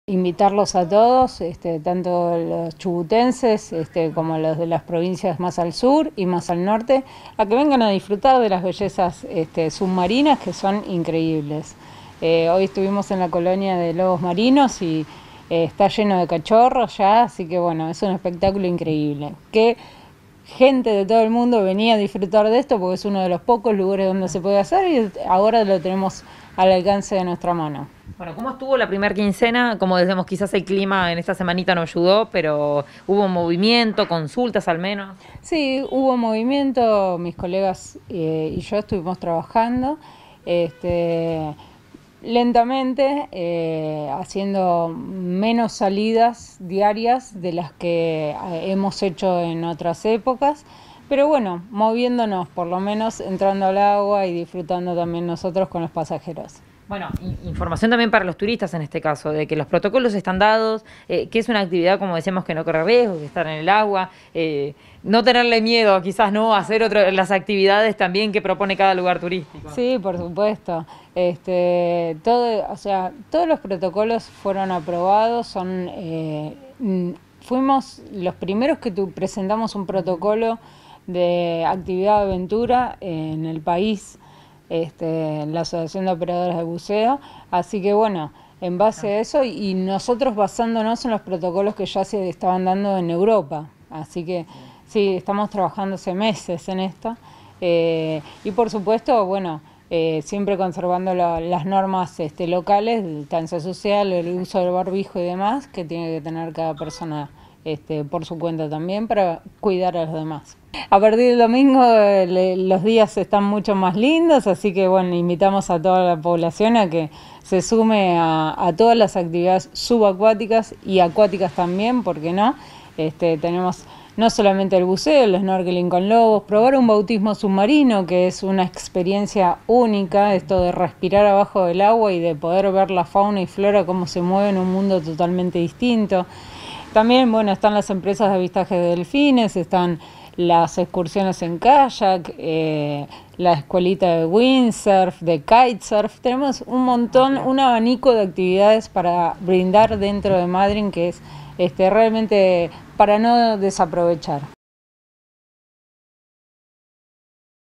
Disfrutar de las bellezas submarinas en Puerto Madryn
Buceo y snorkeling bajo protocolos.
Nota: Carolina Larracoechea- Asociación Operadoras de Buceo.